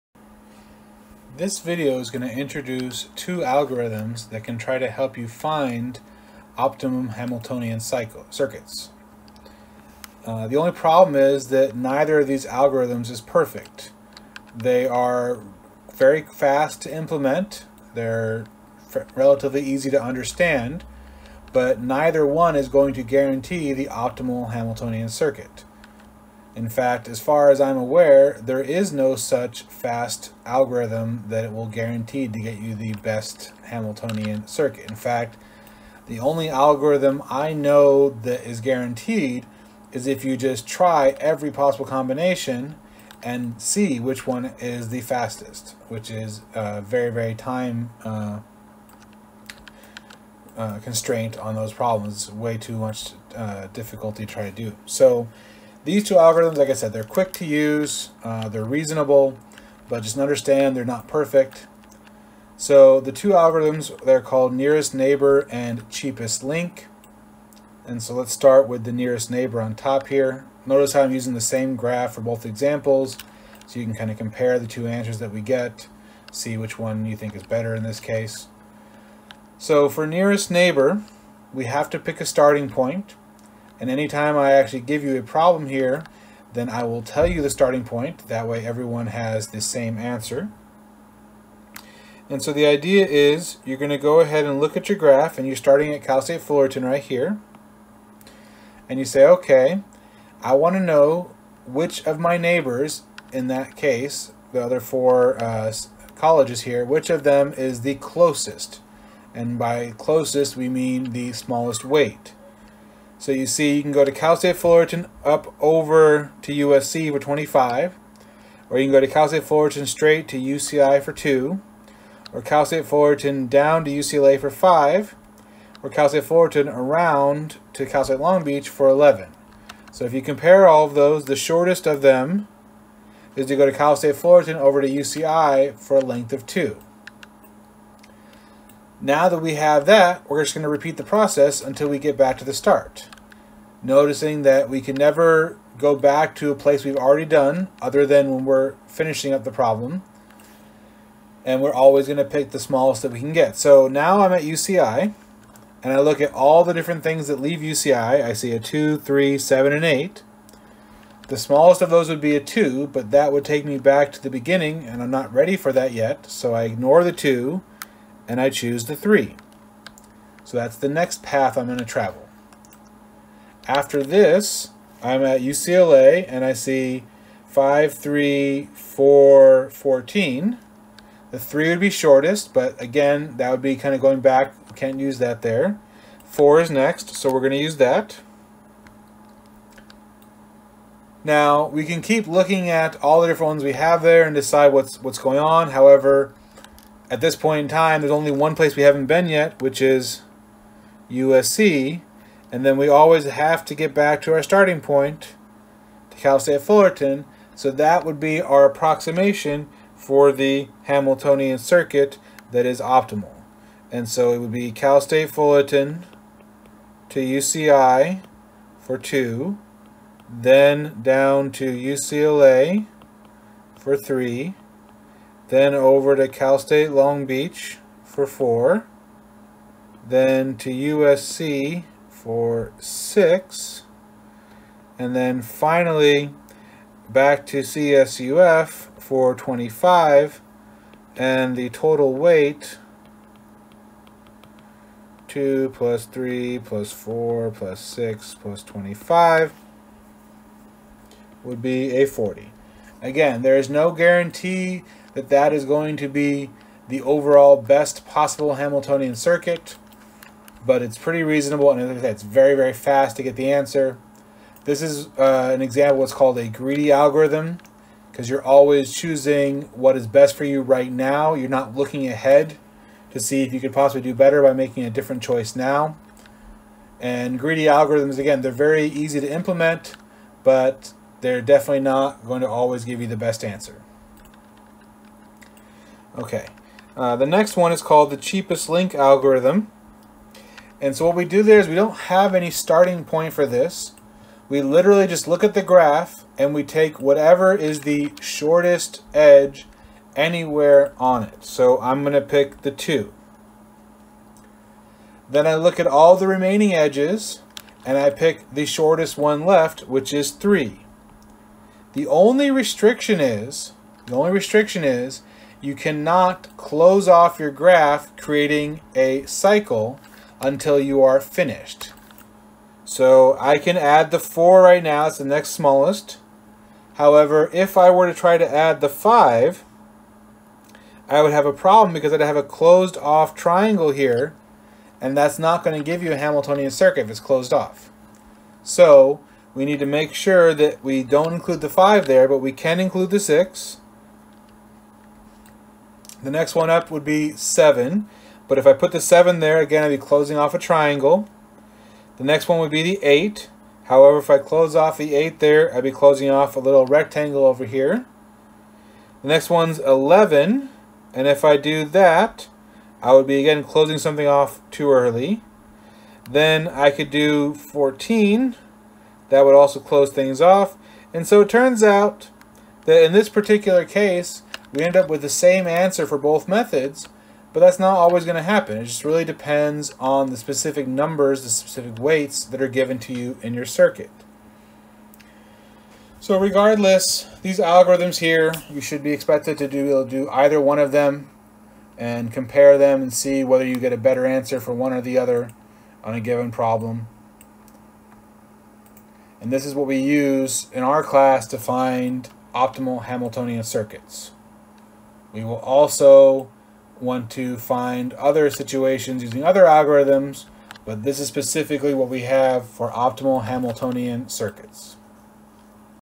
Nearest Neighbor and Cheapest Link
This video goes over the nearest neighbor and cheapest link algorithms to find shortest Hamiltonian circuits.